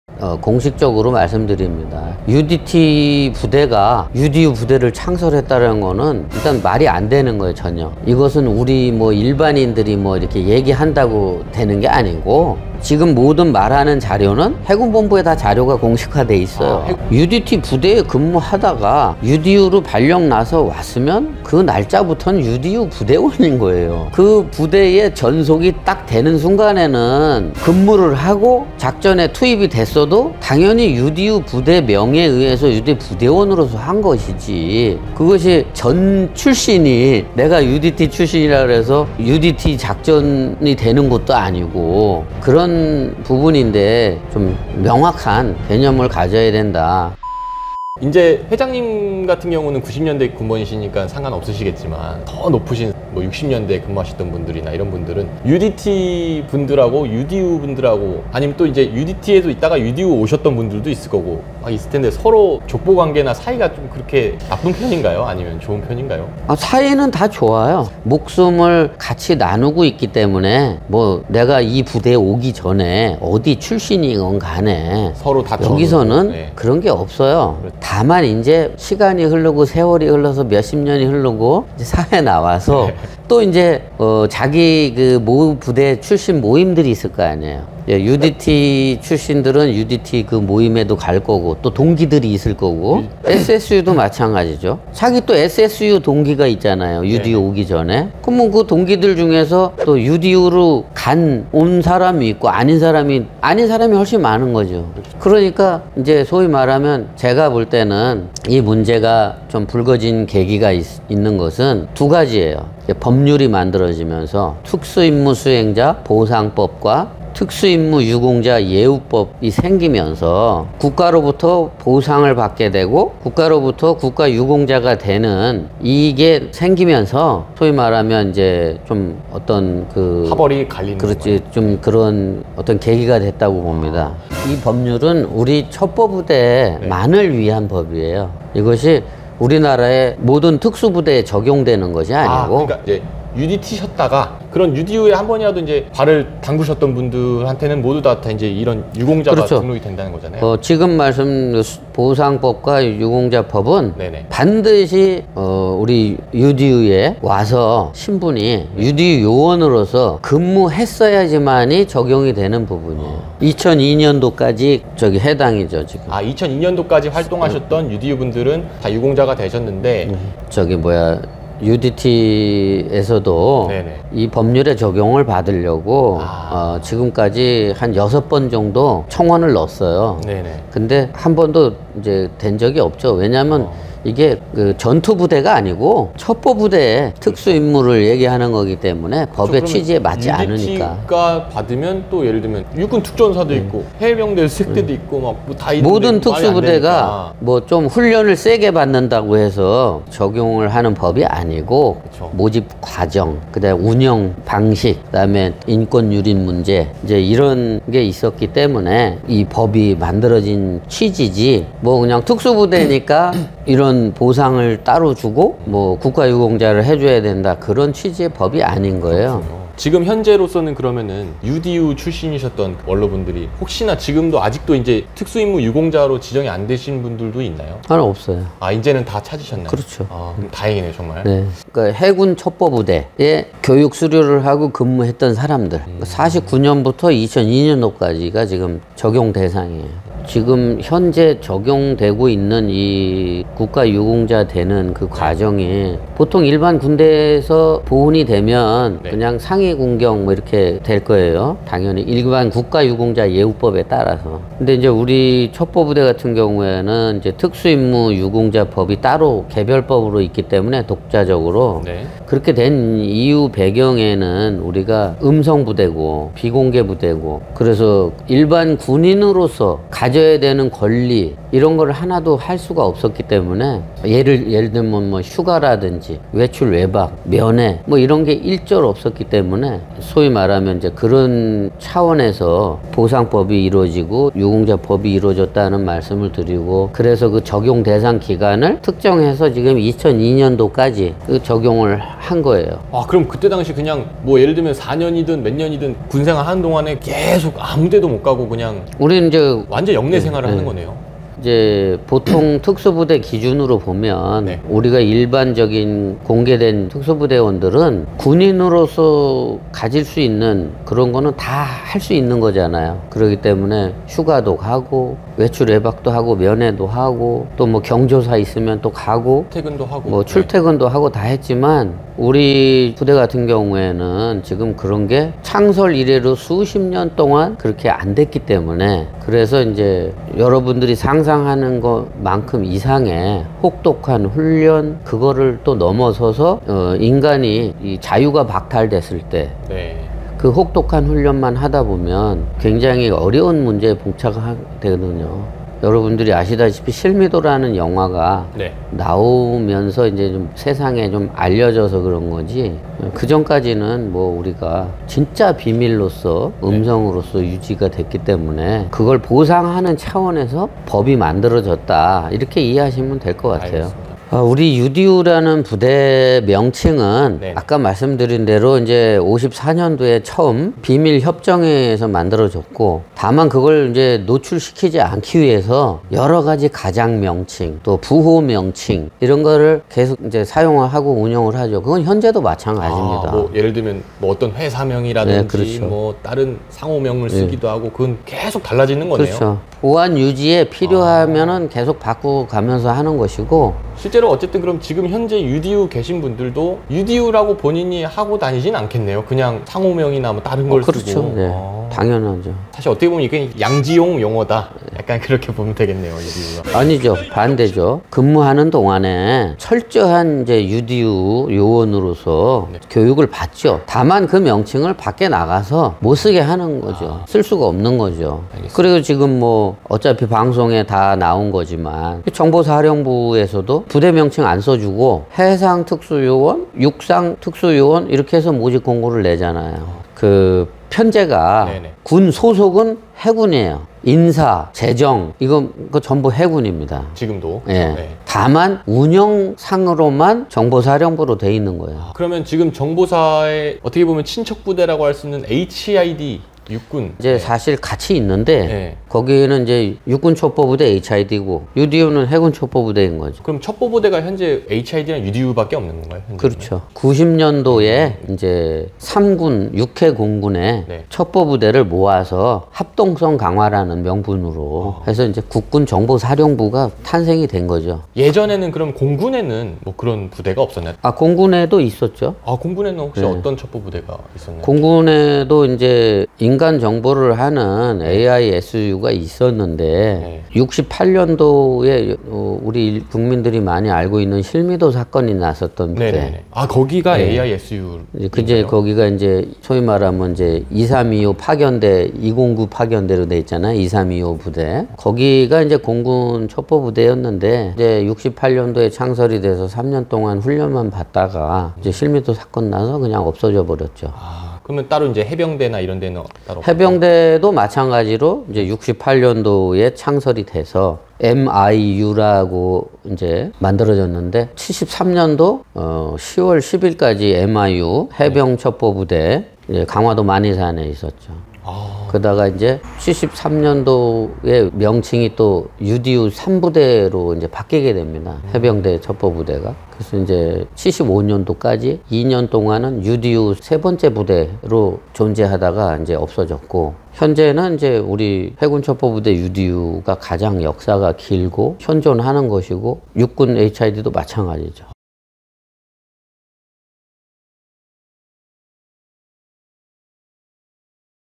"북파공작부대 UDU"를 UDT가 창설했다고 주장하는 이유 [UDU회장 인터뷰 3부]
UDU 예비역 중앙회장님과 인터뷰 3부입니다.
저는 중립기어 박겠습니다. 반론은 언제든 환영입니다 인터뷰 문의주세요.

Q. 해군 UDT가 UDU를 창설했다는 말이 있던데??
Q. 특수부대 예비역들간의 사이는 어떤가요?
Q. 특수부대 예비역간 파벌이 생기는 이유
Q. 첩보부대의 역사